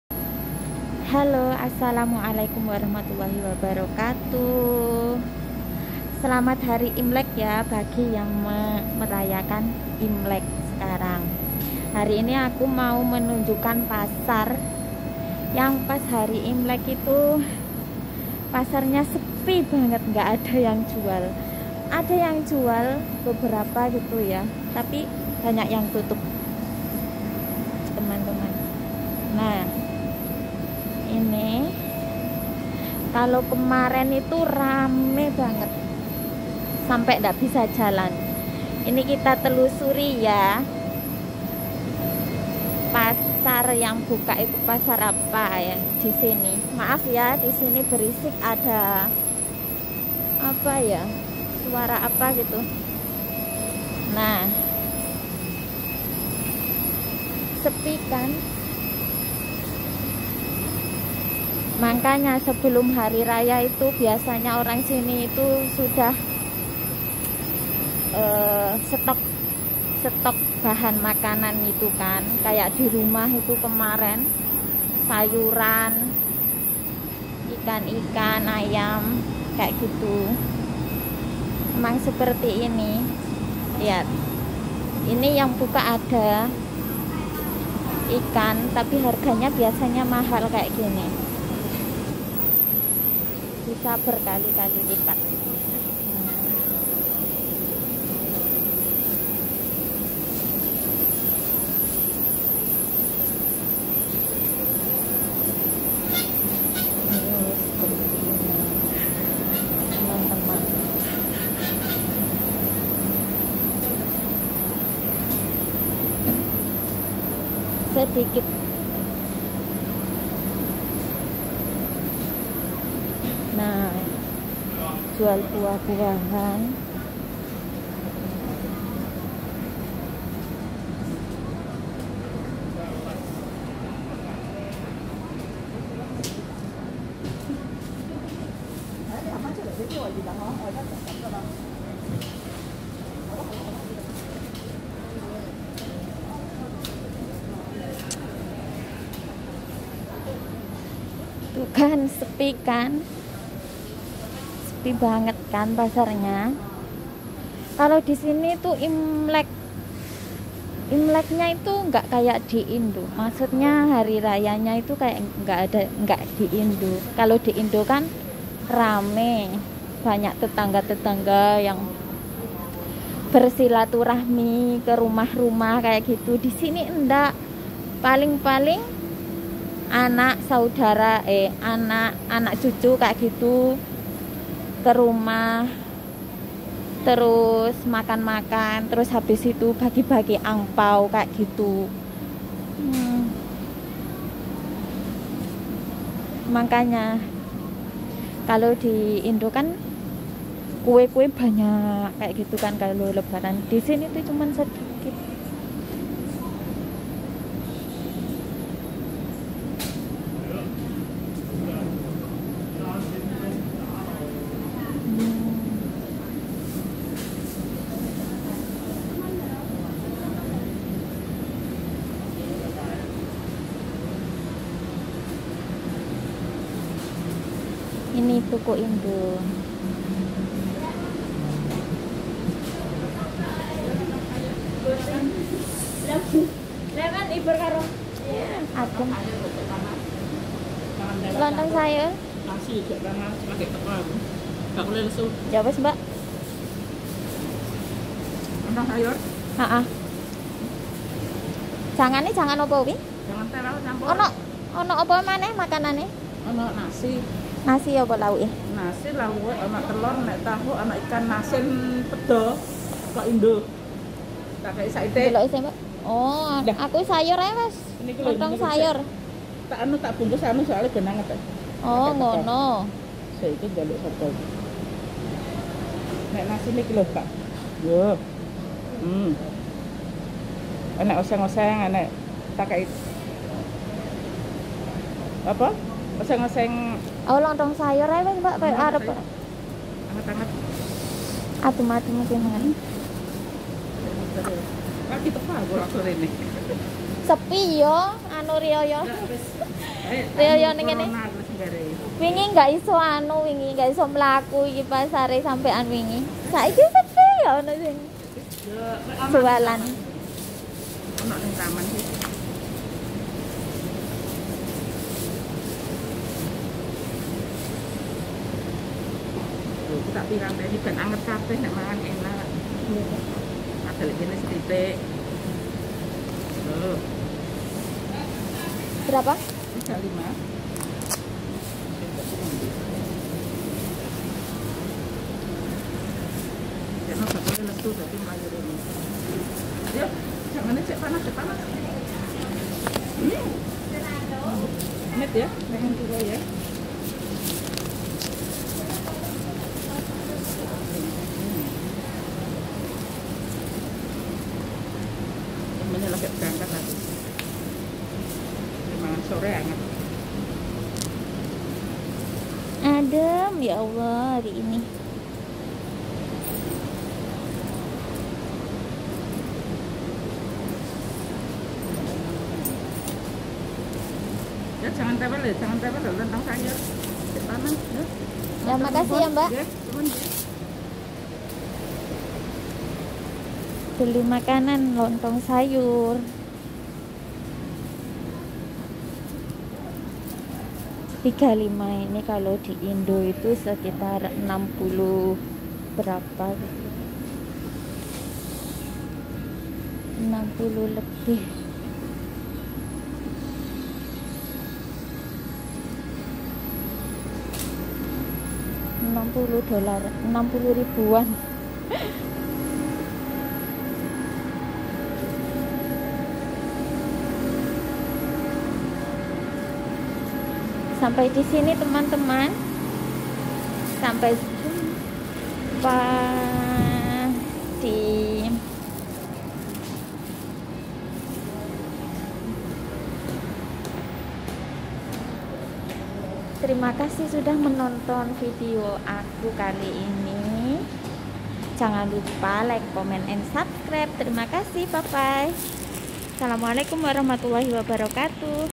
Hari pertama imlek pasar tradisional hongkong sepi baget
bismillahirrohmanirohim assalamualaikum teman onlenku di manapun berada apa kabar semuanya semoga kalian semua selalu dalam lindungan alloh swt amin.

hari pada tgl 1.2.2022 bertepatan hari imlek pasar tradisional di hongkong sepi tidak ada penjual maupun pembeli ada sebagian yg jual tapi dikit baget dan biasanya harganya sagat mahal.

trimakasih sudah nonton fidio aku jika suka like 👍 komen and subcreb trimakasih